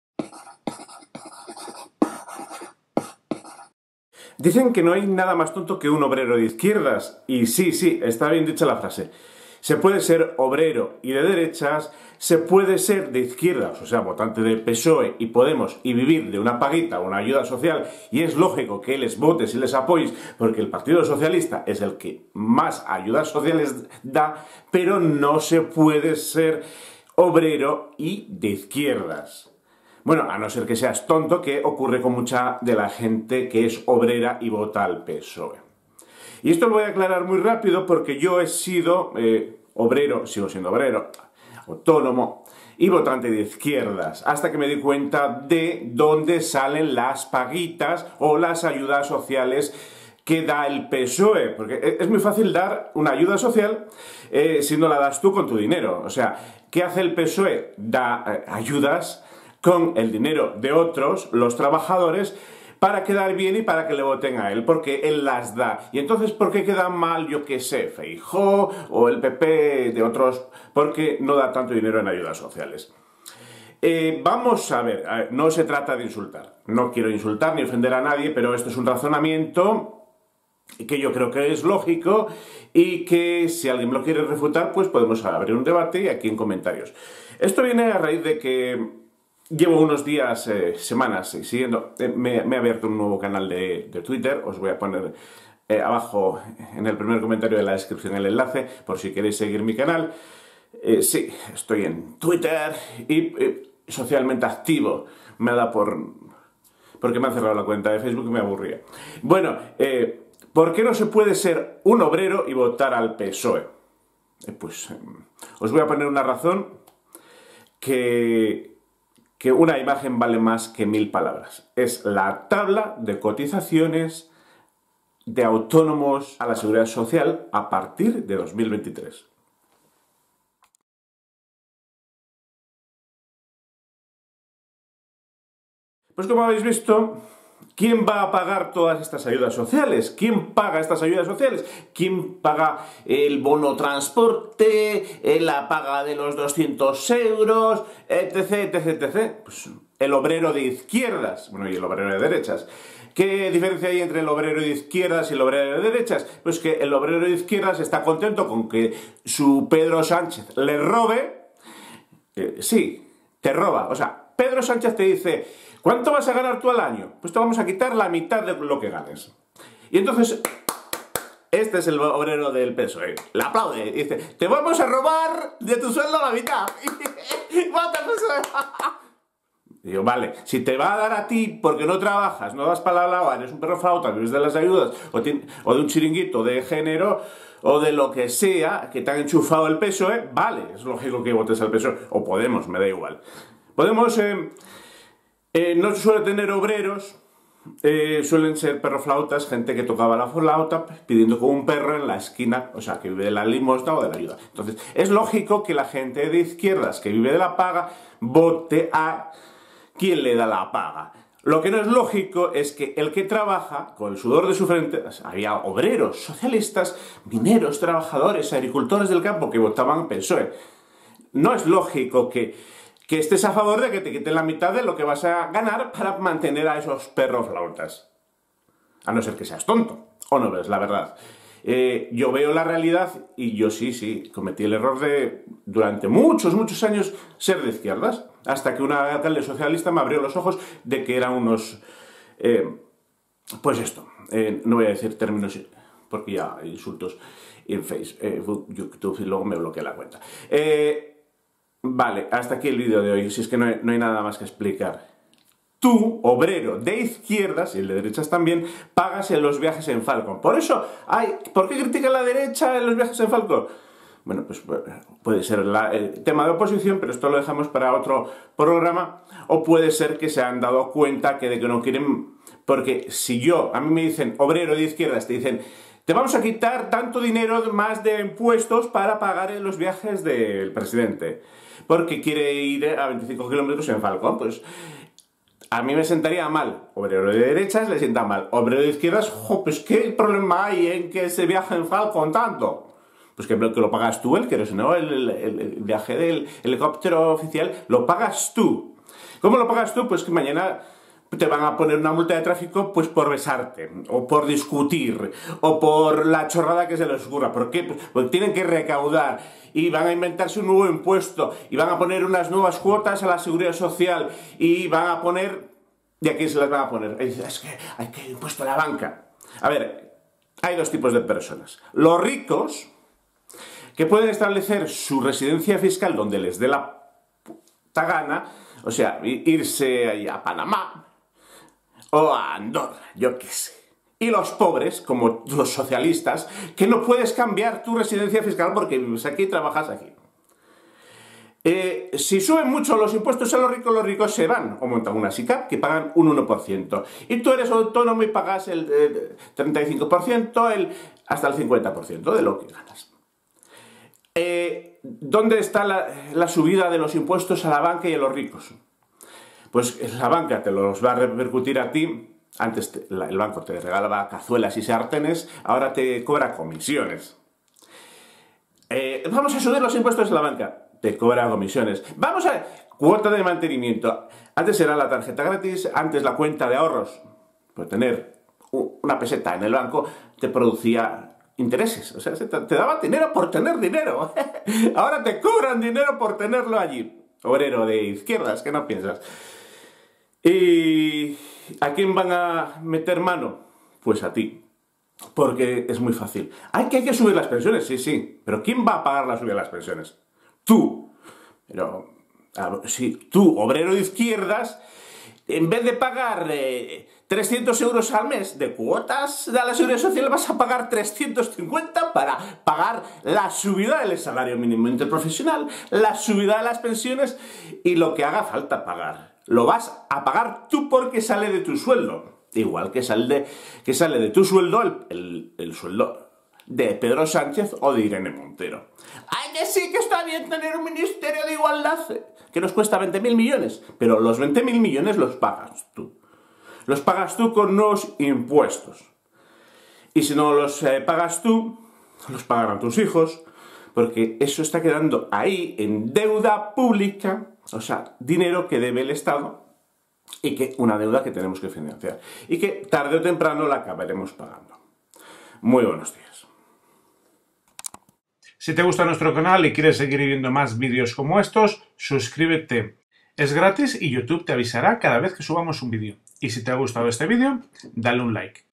Bono tren, bono 200 euros, excepción ibérica ¿QUIÉN PAGA ESO?
La gente de izquierdas cree que ES GRATIS, que la política del Gobierno paga el bono transporte y las ayudas del Gobierno, sin darse cuenta que ese dinero LO APORTAMOS TODOS CON NUESTROS IMPUESTOS